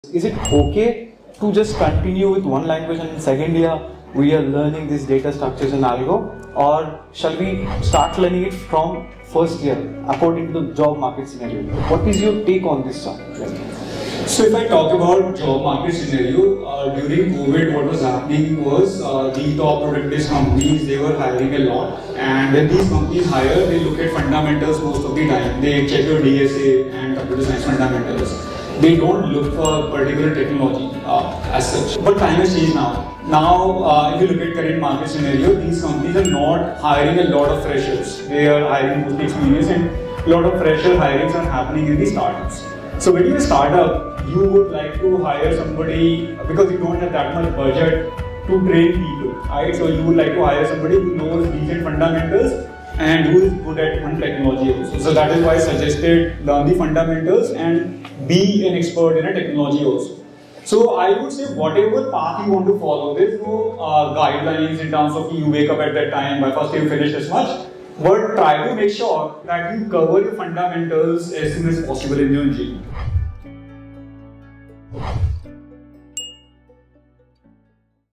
Start DSA Early or Not? College Students Must Watch! | Sandeep Jain | Bose Bhaiya
🧑‍💻 “Should I master one language first or jump into DSA from 1st year?”

In this 🔥 episode from Techfluence Season 3 at LPU, Bose bhaiya sits down with Sandeep Jain sir (Founder & CEO of GeeksforGeeks) to ask a question every coding student is confused about.

🎯 What’s the right timeline for learning Data Structures & Algorithms?

Sandeep sir shares his expert take on when and how to begin DSA, how to balance learning one programming language, and what the job market actually expects from college students.

🛠️ Perfect video for 1st and 2nd year B.Tech students wondering about DSA, placements, and smart career planning.